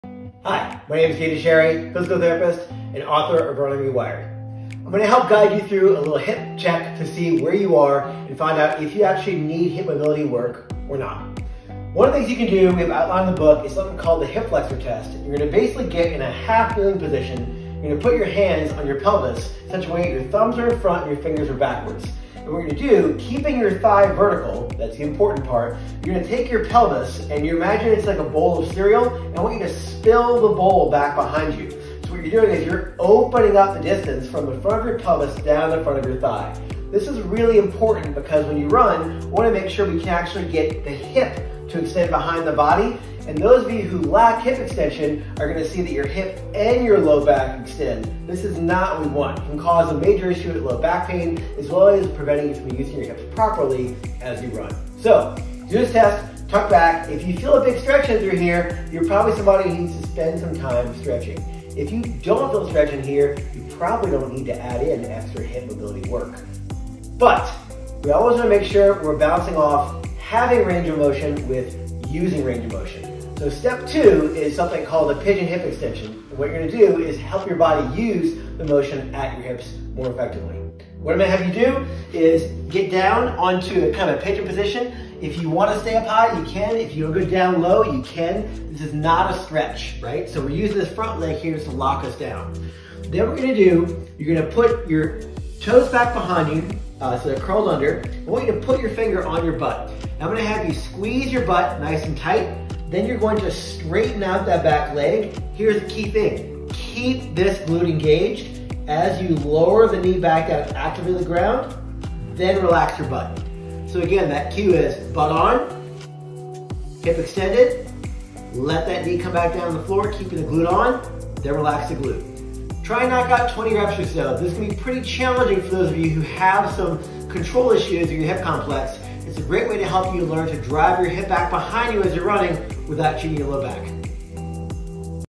Hip Mobility Fix: its not all just stretching
What's MORE important than opening up your hips?USING your hip motion

Sure, lots of people have tight hips that may in fact need stretching. But you need to be able to drive your hips independent of your spine.

Stretching to improve your tissue length won't help you use that new mobility in your daily life.

To use your hips mobility, you need to USE your hip mobility!
Here's a little snippet of the hip flexor test in my book RunningRewired, and a primer on how to use the Pigeon Hip Extension drill to build solid control

If you DO this test, and feel significant tightness in the front of your hips, aim to do this kneeling stretch for 3 min, about 4-5 days a week until tightness subsides. And always follow it with 20 reps of Pigeon Hip Extension drill to help you USE your hip mobility.

If you don't feel much of a stretch, I still recommend the Pigeon Hip Extension drill for a few sessions to build the muscle memory to improve your performance in your sport!